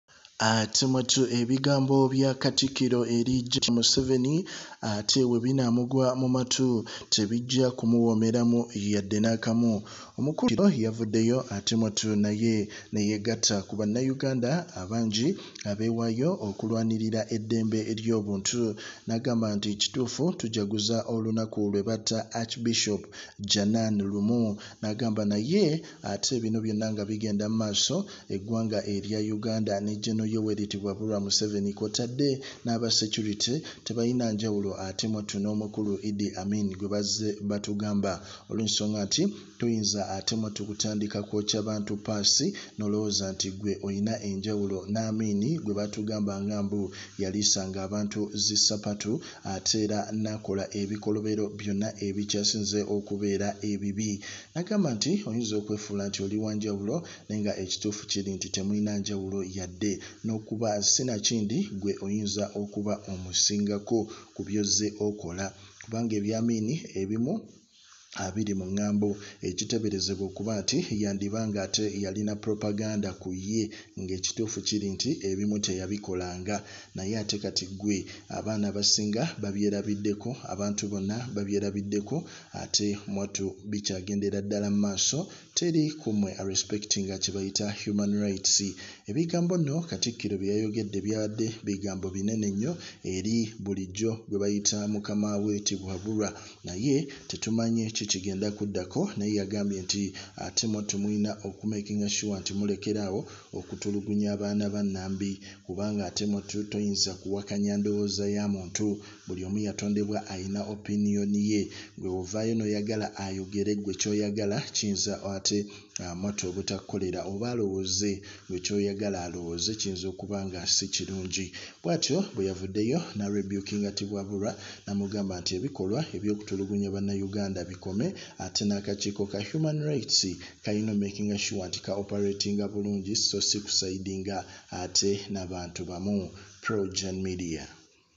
Tukooye! Museveni wanawulira bino mayiga byamwogeddeko Ayinza Osibira mu safe house Buno bunkenke
projournmediatv It’s A Shame!
POLITICSIt’s A Shame! Katikiro Mayiga Rebukes M7’s Gov’t For Torturing Ugandans
Buganda prime minister (Katikiro) Charles Peter Mayiga has rebuked president Yoweri Museveni’s government for torturing Ugandans and called for respect of human rights.
Mayiga lashed out at Ugandan government for commemorating the murder of Archbishop Janan Luwum by Idi Amin yet state security operatives replicate the same.
It is a contradiction and shame that we commemorate the murder of Archbishop Janan Luwum by Idi Amin when state security operatives torture Ugandans in this manner today,” he said.

Mayiga called for respect of human rights saying it is the foundation for democracy and progress.

Buganda’s prime minister attached a picture of National Unity Platform (NUP) supporter Eric Mwesigwa to his message. Mwesigwa was allegedly abducted and tortured by security operatives.